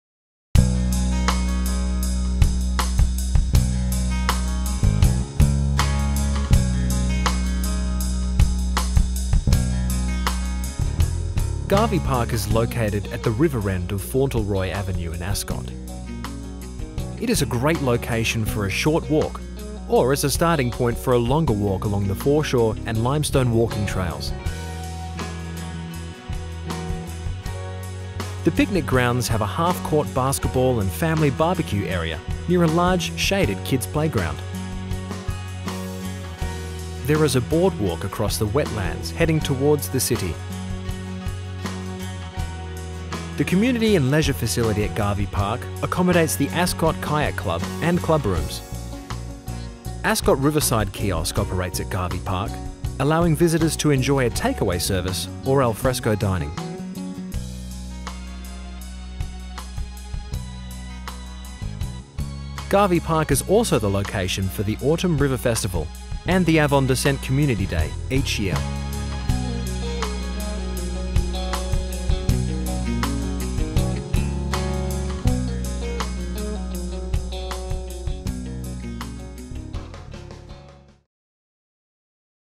Garvey Park | City of Belmont
Garvey Park is located at the river end of Fauntleroy Avenue in Ascot, WA. It is a great location for a walk along the Swan River foreshore. Spot local wildlife, have a picnic or bbq, let the kids play in the playground or have a game of basketball on the half court. The park is home to Ascot Kayak Club and the Ascot Riverside Kiosk